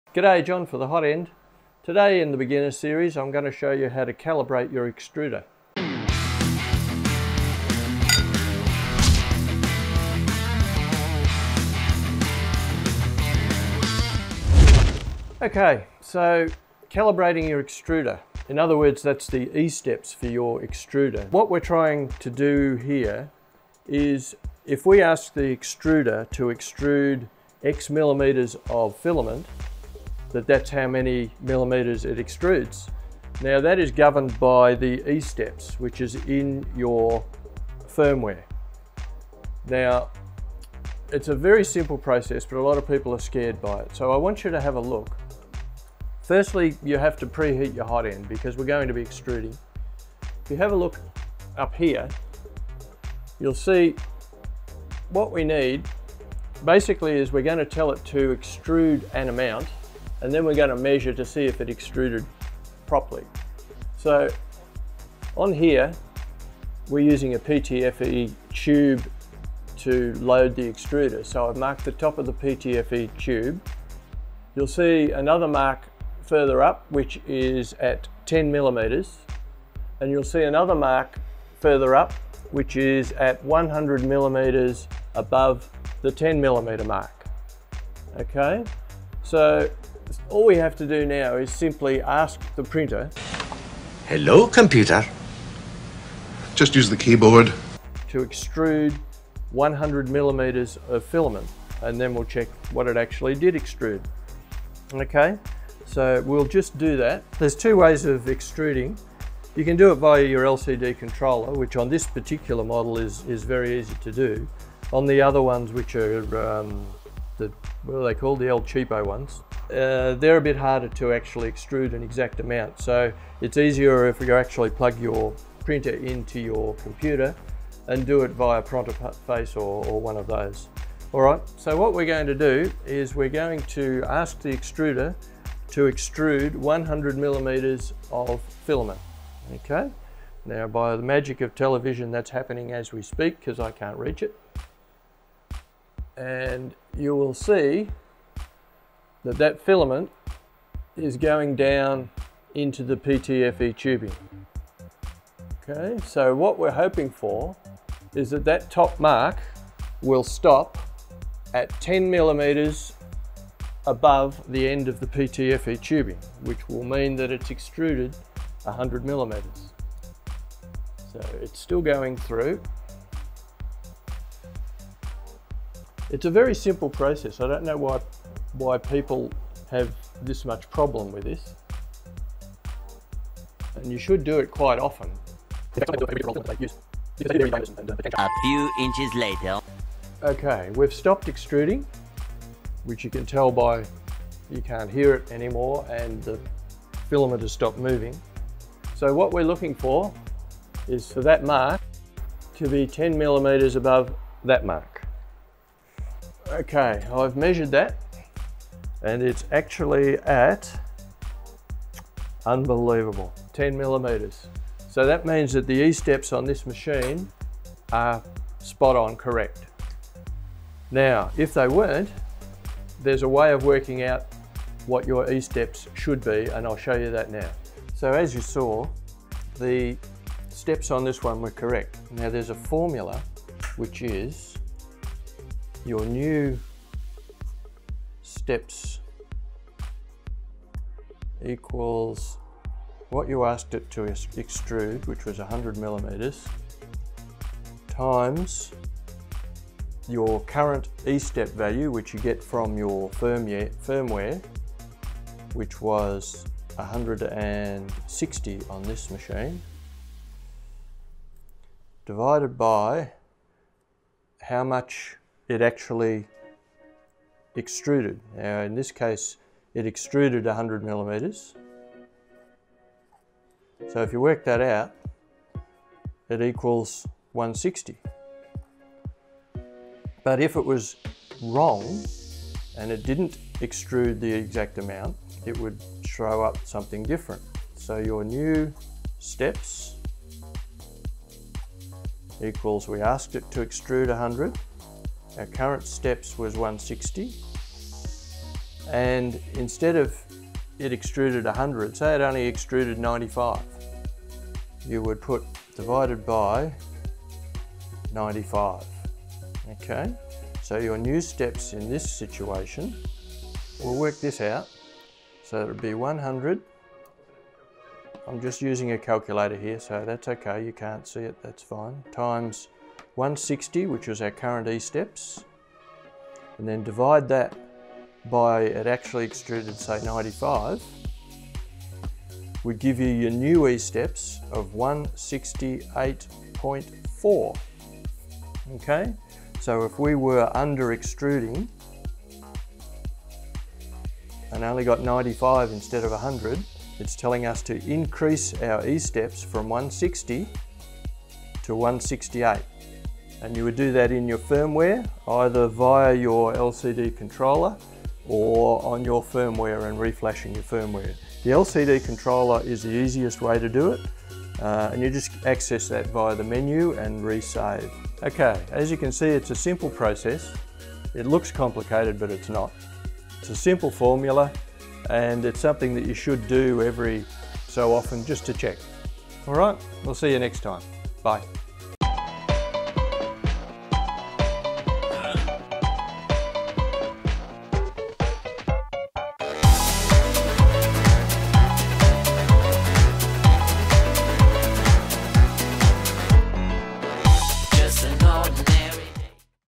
How to Calibrate the Extruder E-steps on your 3D Printer 🔧
This is part of our back to basics series.

In this video John will go through the process of testing your e-steps for your extruder. He then goes through the maths how to get the exact e-step for your printer! Leading to Much better printer without under or over extrusion.

Part two will be applying our newly found values in smoothieware and marlin.

👐  Do you run an AdBlocker? Consider donating to Help Out 👐

▼▼ Postage Details ▼▼
AUSTRALIA